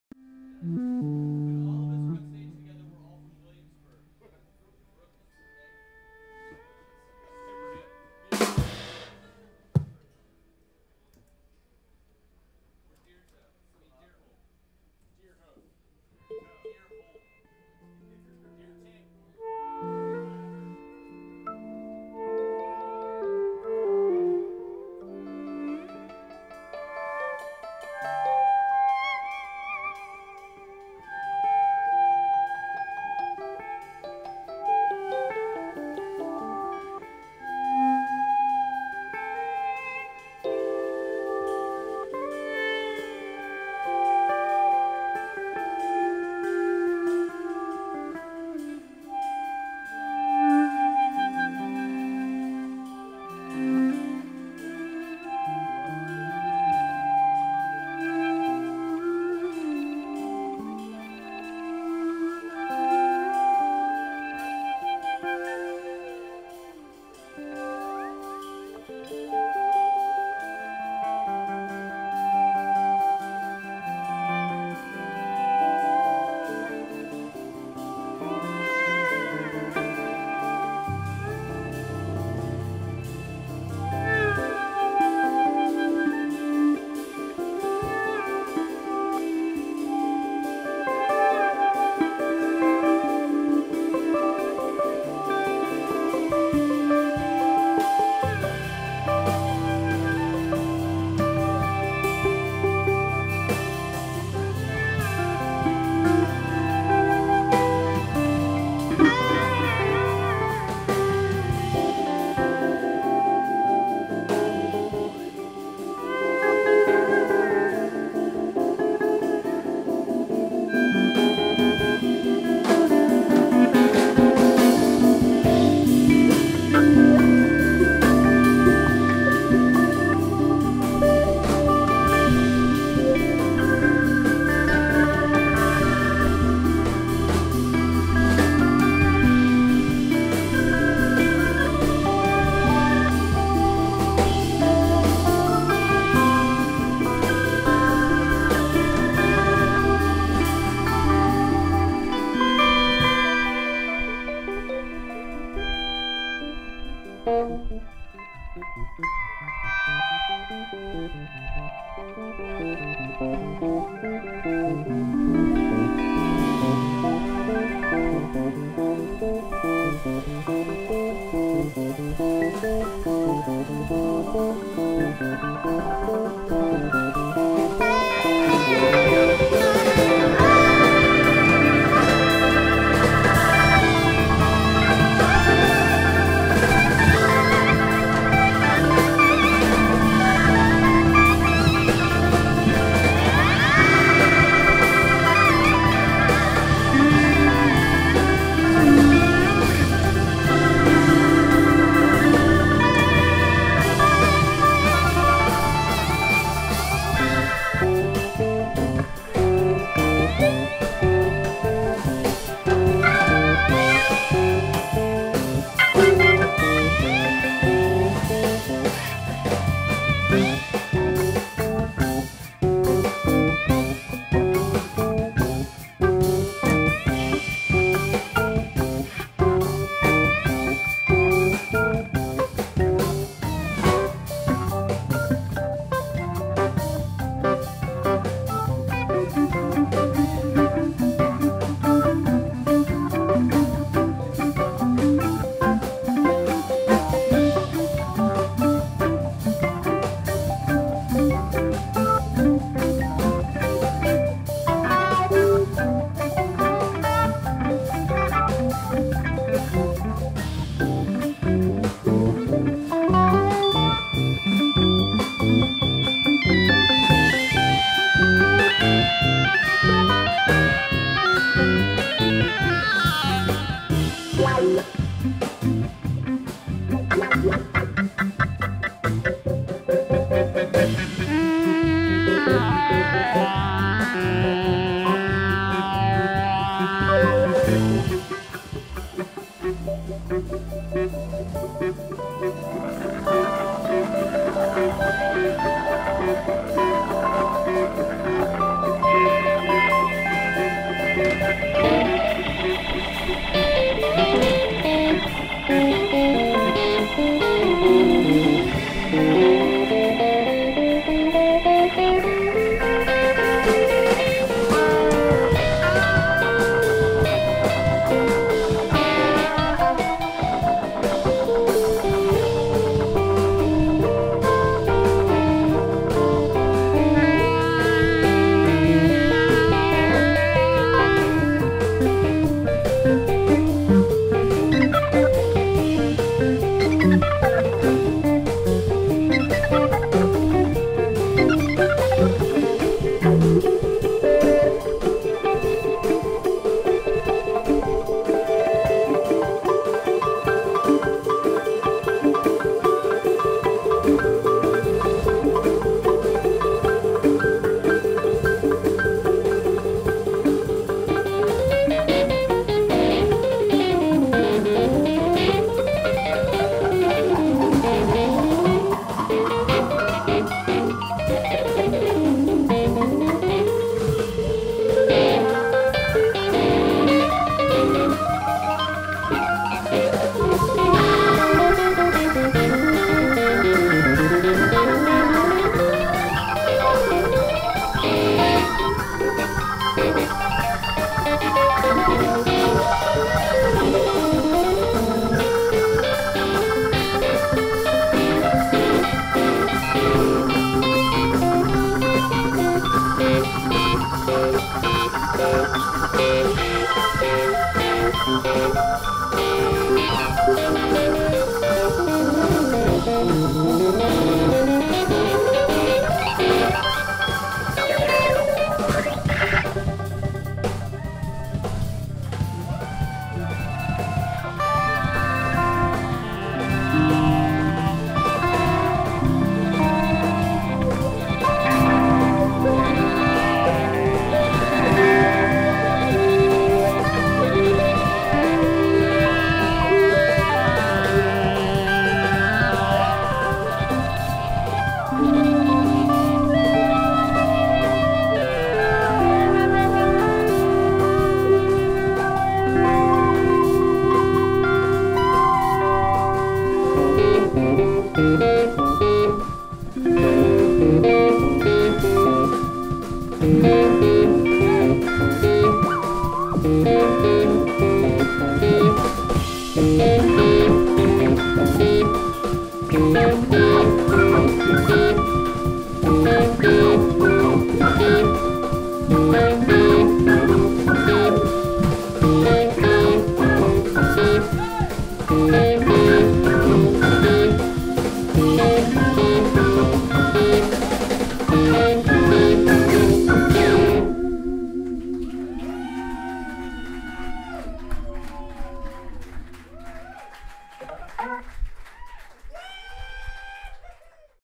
Bounce/Betamax Jam Round Robin Grudge Match 10-24-14, Duende, Oakland CA
Billled as Round Robin Grudge Match with Jacob Fred Jazz Odyssey , McTuff and Amendola vs Blades on 2/23-24/14
Brian Hass, Joe Doria, Andy Coe, Tarik Abouzied, Chris Combs
Video: Sony Rx100mII
Audio 2Jospehson C42MP & 2 Telefunken M81 microphones close mic'ed